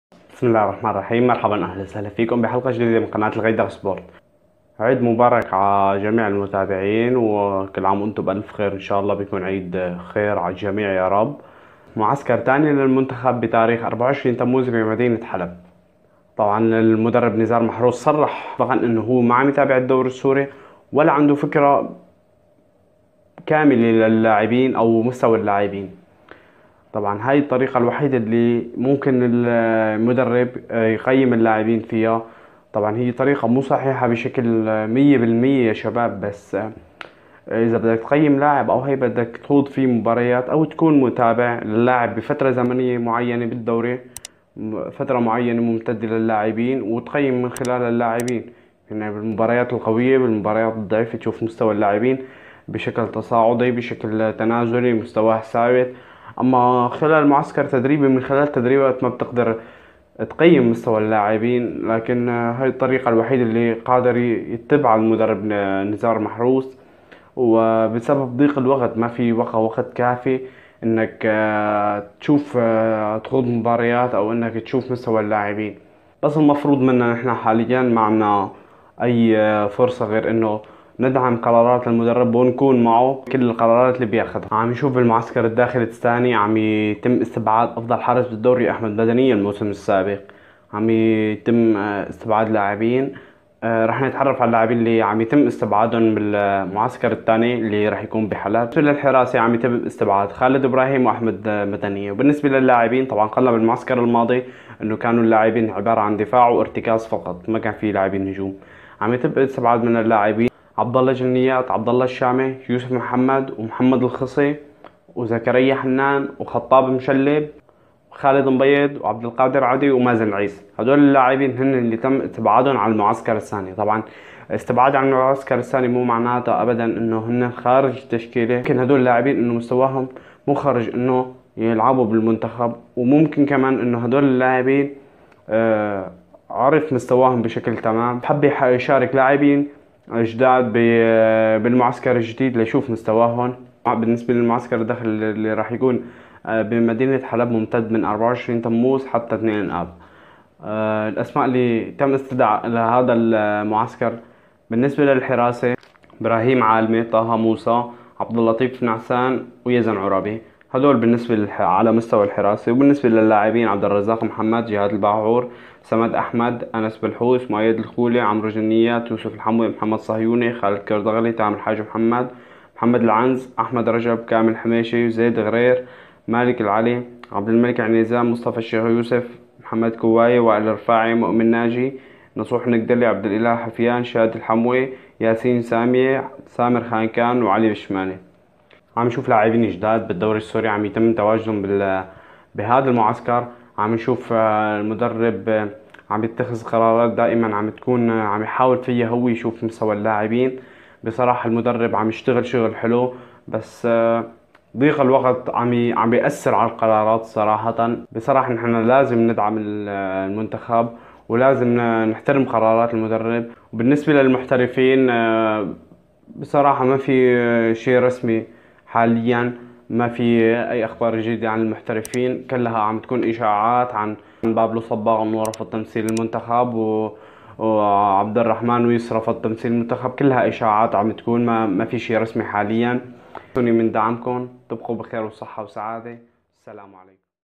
معسكر داخلي ثاني للمنتخب السوري واستبعاد المدنية!!! | وكل عام وانتم بخير
أخبار المنتخب والمعسكرات والتحضيرات.
المعسكر الثاني للمنتخب وموعد المعسكر
اسماء اللاعبين يلي تم استبعادون عن المنتخب السوري وشو عم يعمل الكابتن نزار محروس
ومين اللاعبين يلي رح يتواجدو بالمعسكر الثاني.
كل تفاصيل المنتخب موجودة بالفيديو
رابط صفحة الفيس بوك الغيداق سبورت Alghaidak sport :